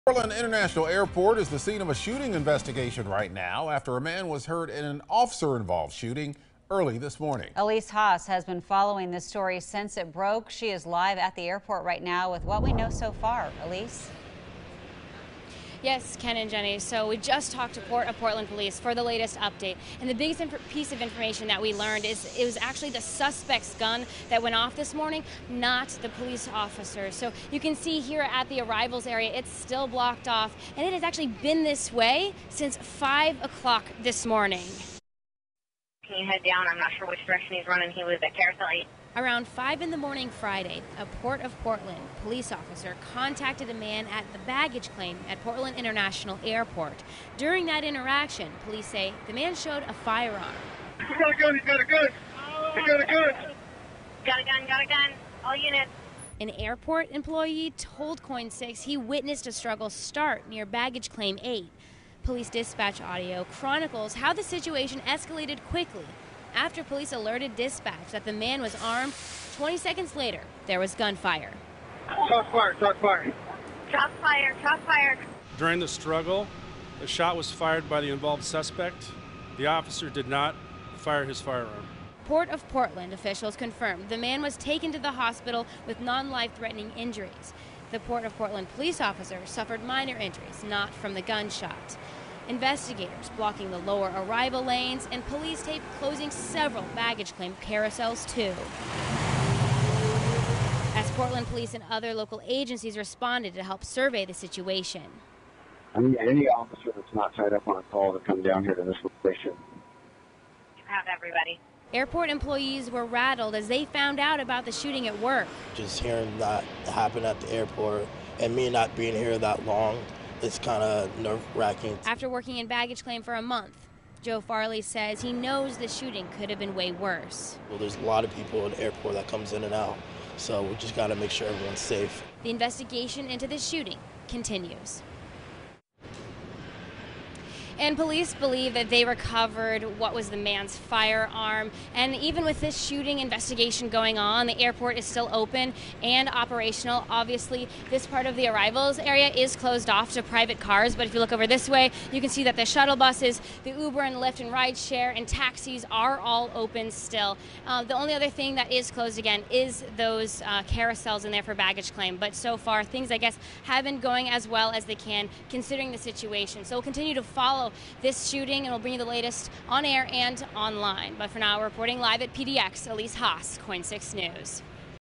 Man shot at Portland Airport
A gun went off during the struggle and injured the suspect.